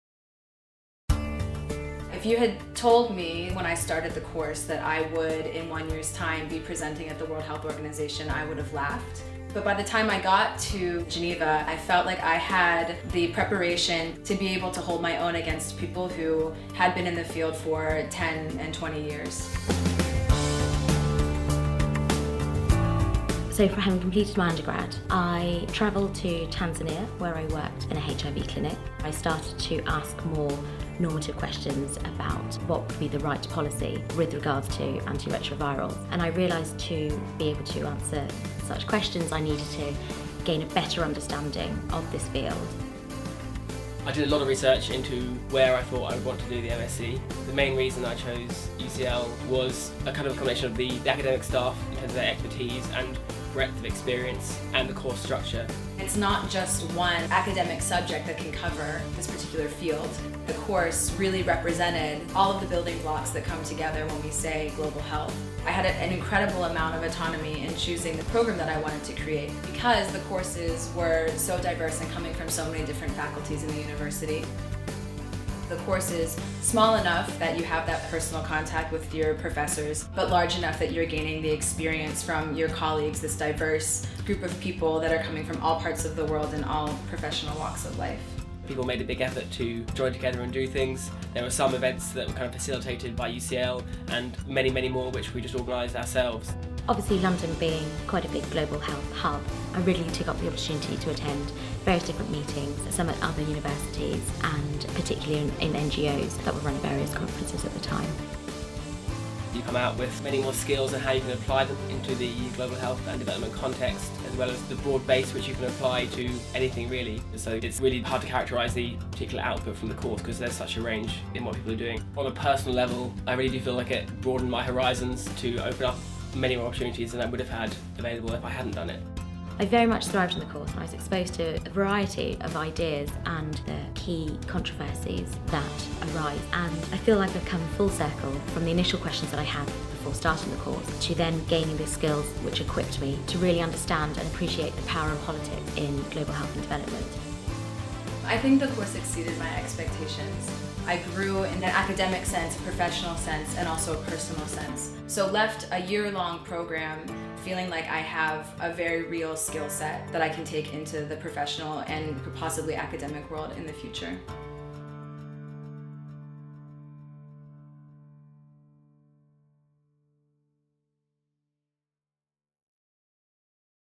MSc Global Health and Development at UCL
The MSc in Global Health and Development at UCL will challenge you to understand the complex forces that shape health worldwide, from the household to the global level, with the opportunity to craft a programme of study well-suited to your needs.

Produced by Foolio Films

The UCL Institute for Global Health is an initiative to garner the power of UCL to address the most pressing issues of global health today.